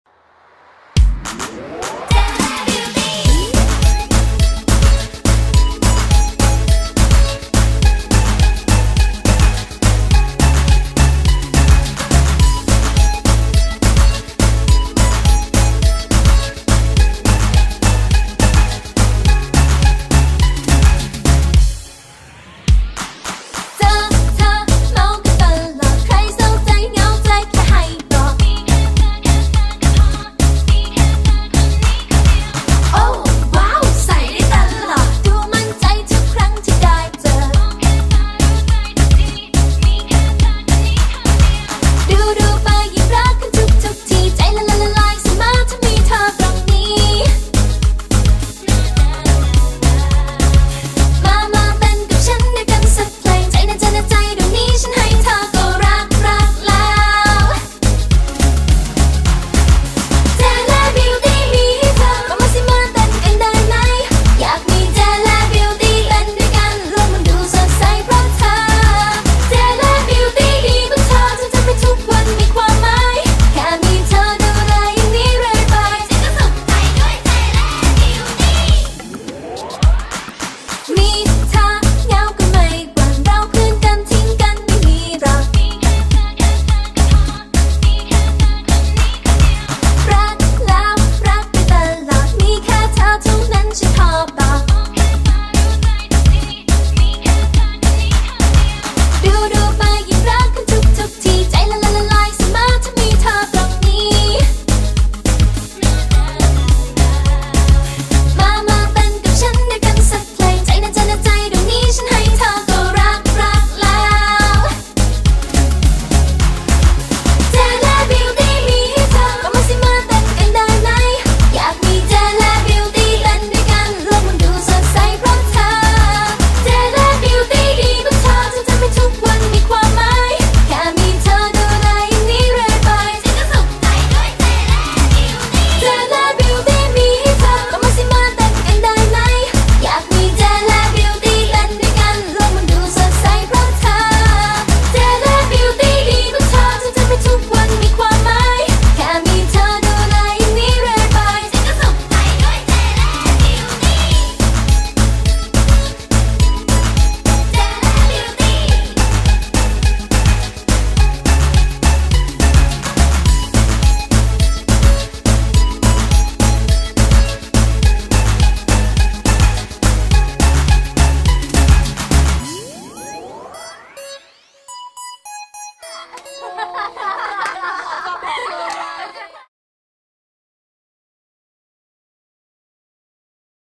Hello! Jele
คลิปส่งประกวด โครงการ Jele Jeli Dance ได้รางวัลชนะเลิศประเภทสวยงาม